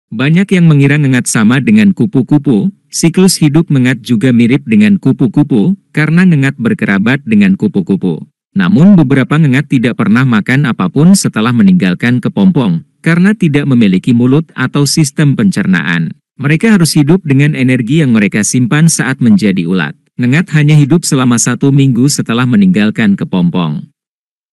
hewan ini tidak makan selama hidup
serangga ngengat moth tidak makan selama hidupnya